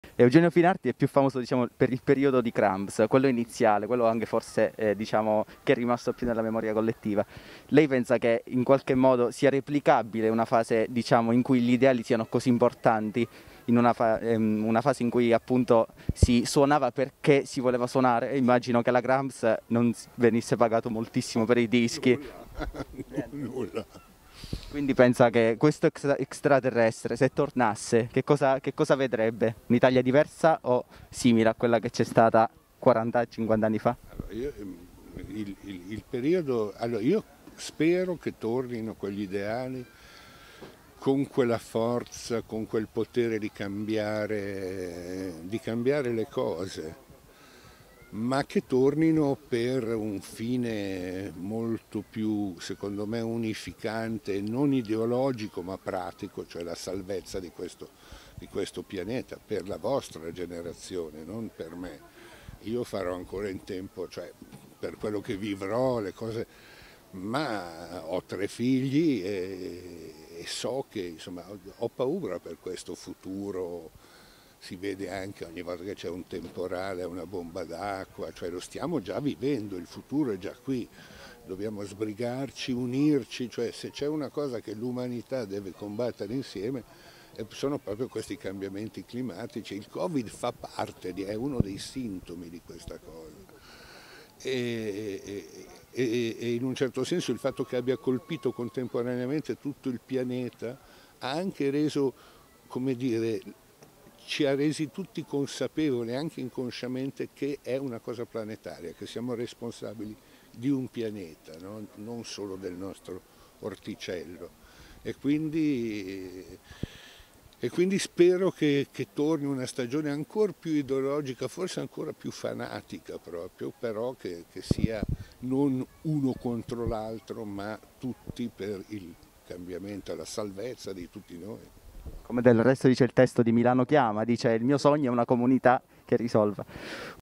Eugenio Finardi sul Covid
Eugenio Finardi esprime le sue posizioni sul Corona Virus. Intervista fatta da me il 4/09/2020 a Teramo.